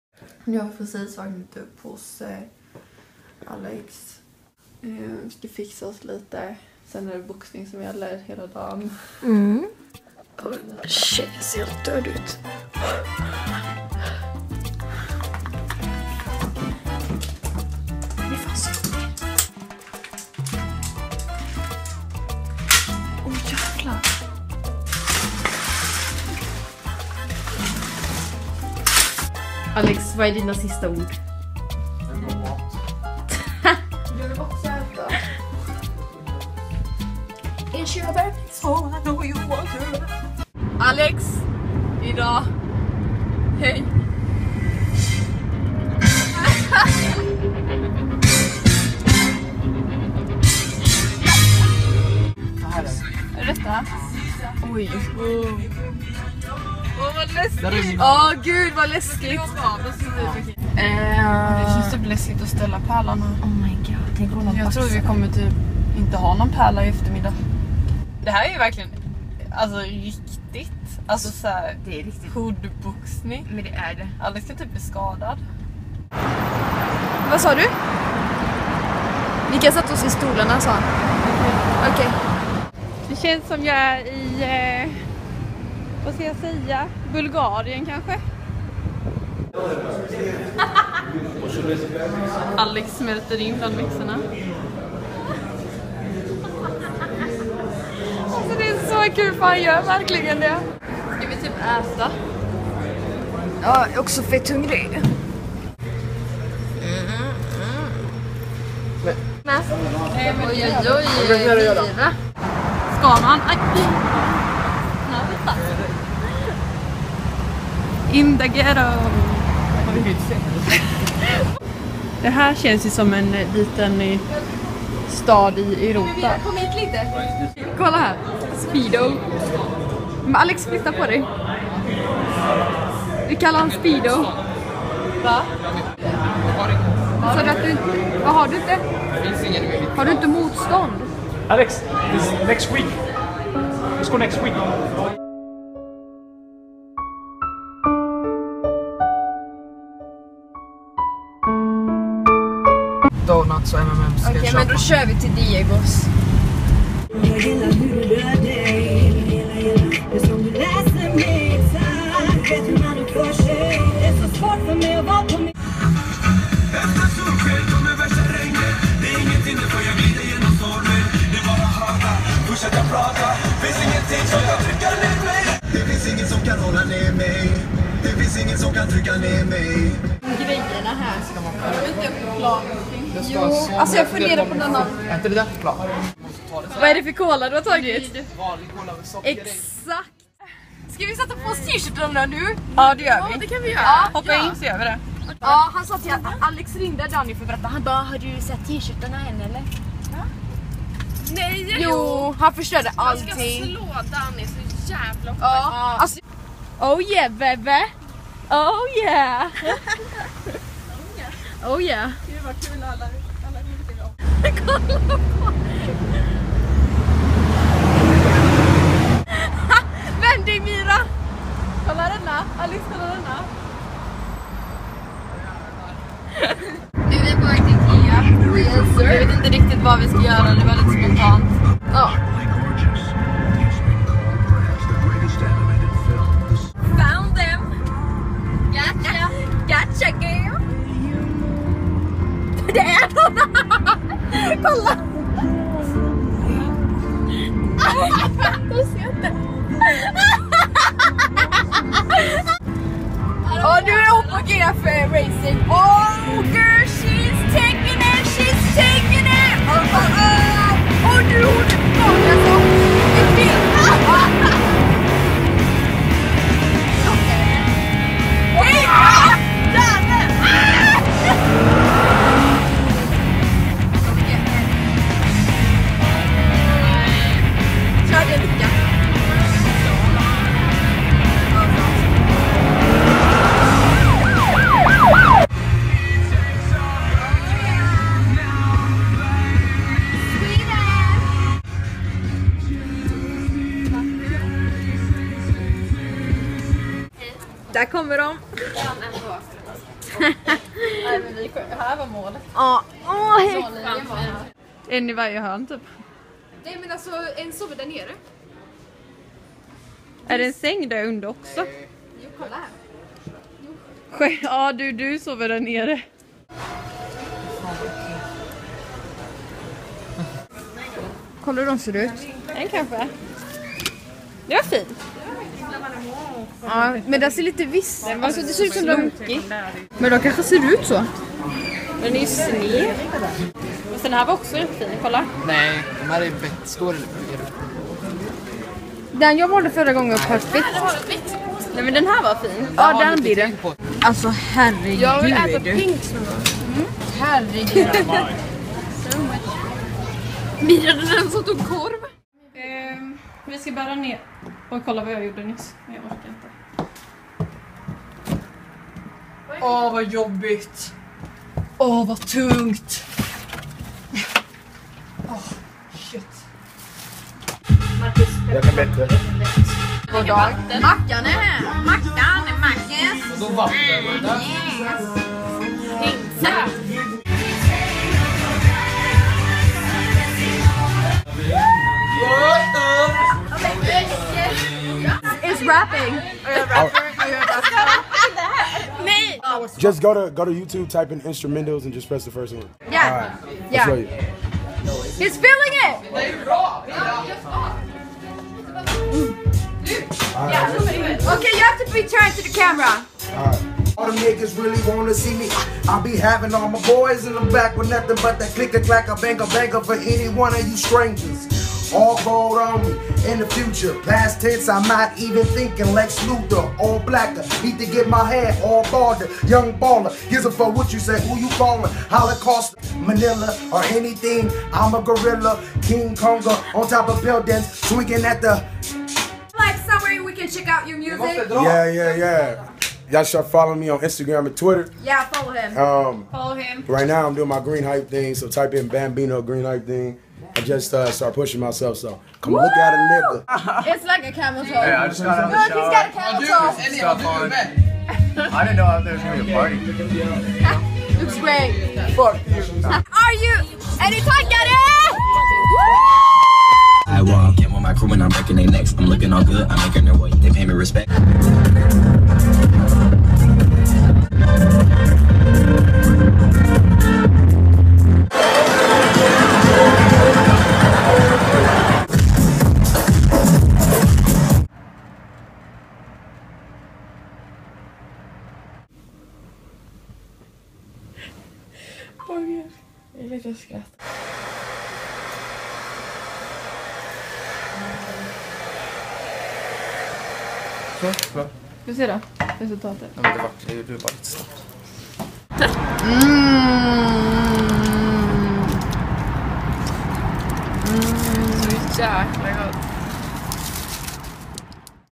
Vlog 17 - Alex Boxningsmatch, Racing och Utgång
Vardagen som student på Santa Monica College, i Los Angeles, Kalifornien.